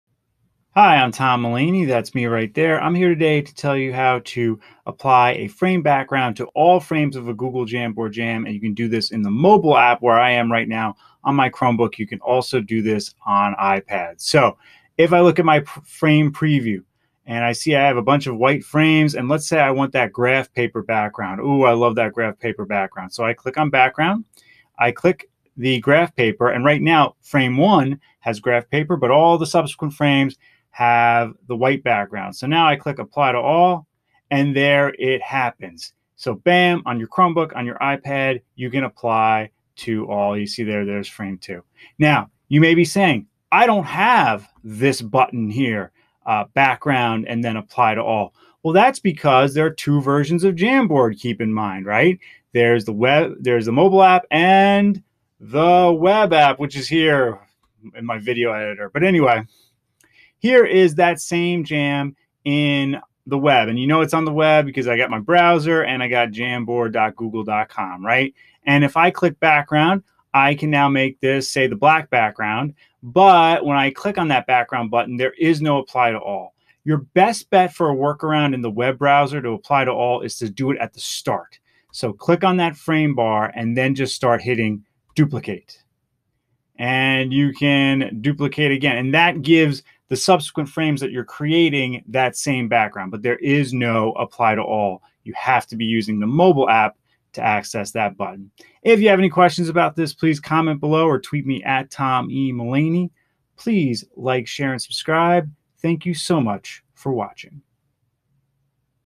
Apply a Frame Background to All Frames in the Jambord Mobile App
Here is how to apply a background to all frames in the Google Jamboard mobile app for Chromebooks and iPads.